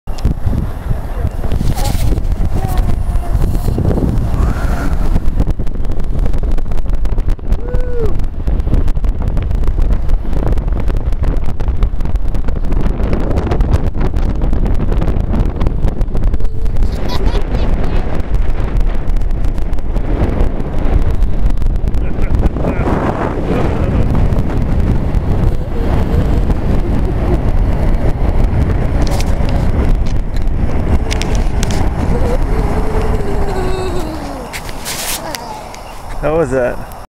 Ben tubing at Gorgoza Park
Ben saw snow for the first time in his life yesterday.  Today he got to ride the tubes at Gorgoza Park, just west of Park City.  Fun day!!!